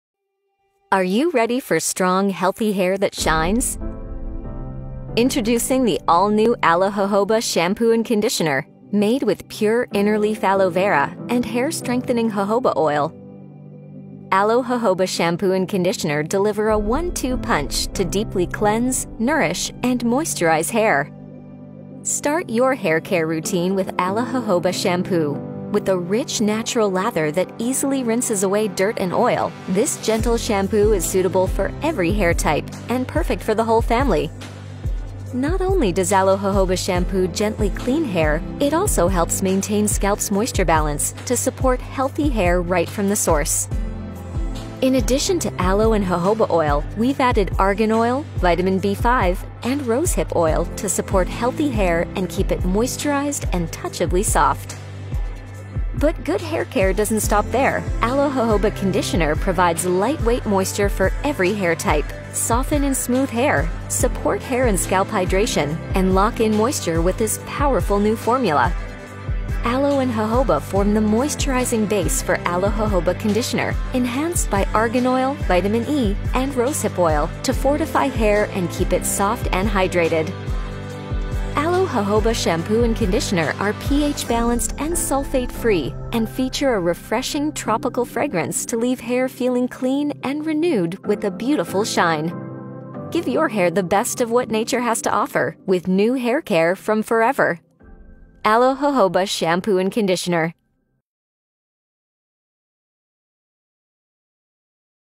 Aloe Jojoba Shampoo & Aloe Jojoba Conditioner | Forever Living | Muhammad Sarim
Nourish your hair with Forever's Aloe Jojoba Shampoo and Aloe Jojoba Conditioner. Made with pure inner leaf aloe vera and hair strengthening jojoba oil, this powerful pairing is perfect for everyday use for soft, manageable hair with stunning shine.